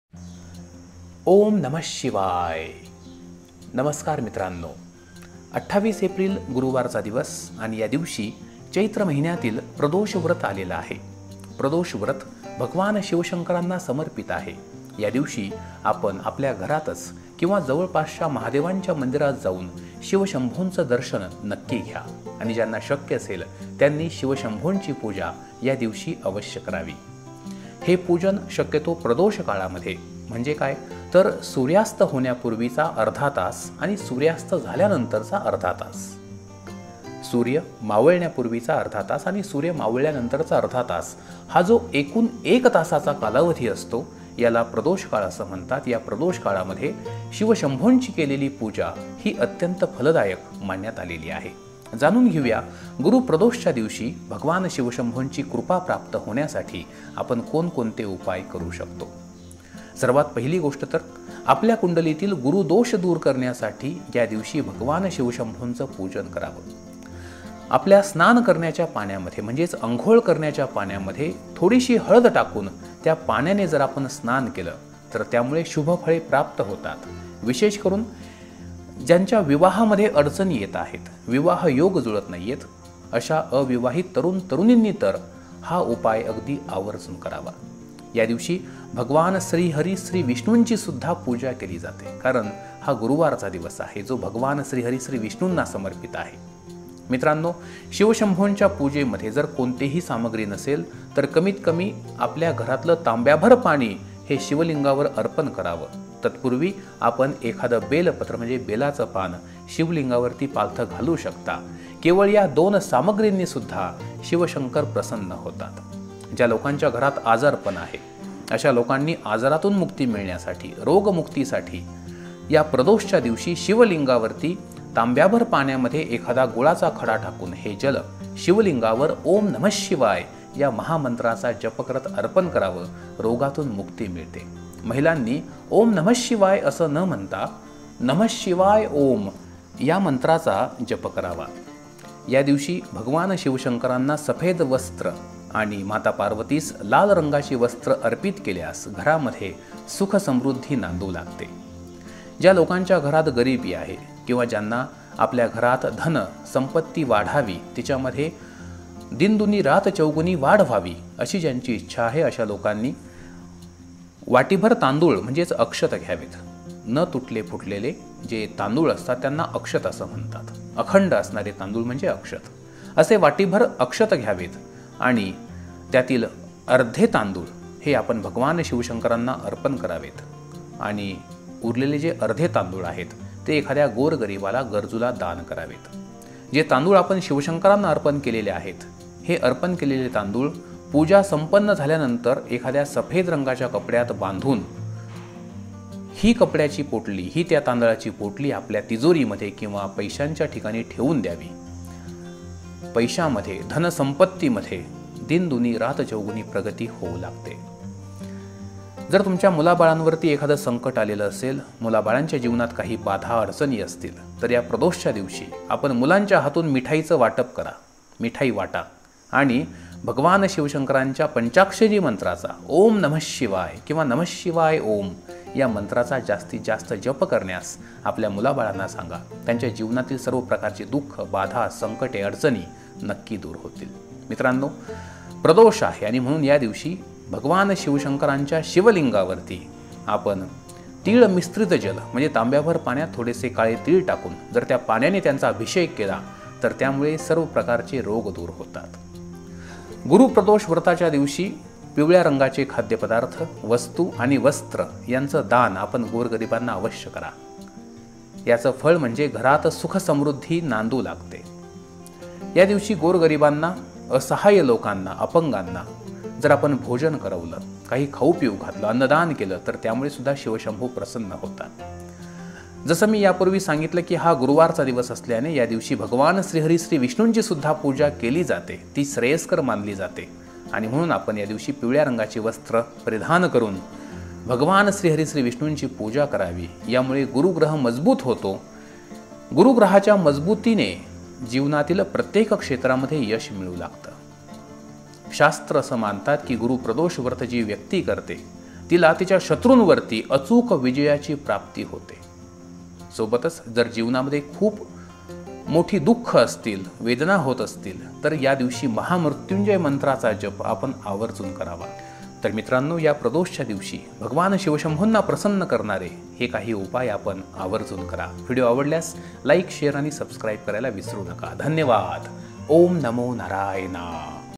28 एप्रिल मोठा गुरुवार (प्रदोष) गुपचूप ठेवा इथे मूठभर तांदूळ, पैसा इतका येईल की सांभाळता येणार नाही.
28 एप्रिल मोठा गुरुवार (प्रदोष) गुपचूप ठेवा इथे मूठभर तांदूळ, पैसा इतका येईल की सांभाळता येणार नाही.